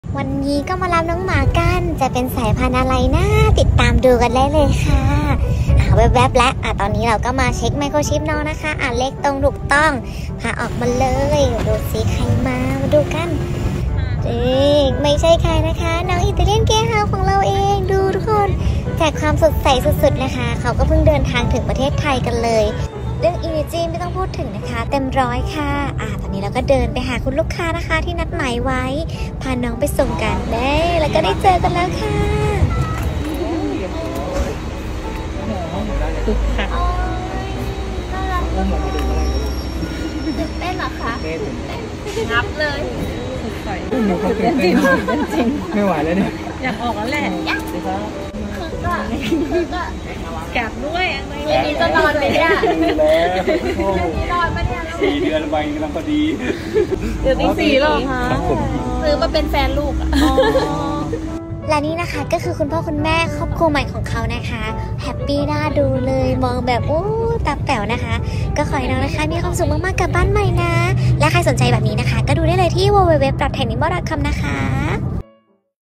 Italian greyhound นำเข้าจากเกาหลีมาแย้วค่า ✈️✨🫶🏼
ถึงไทยแล้วค้าบบ ป่ะะ พามารับเด็กเกาหลีกันน้าา 🐶🫶🏼❤️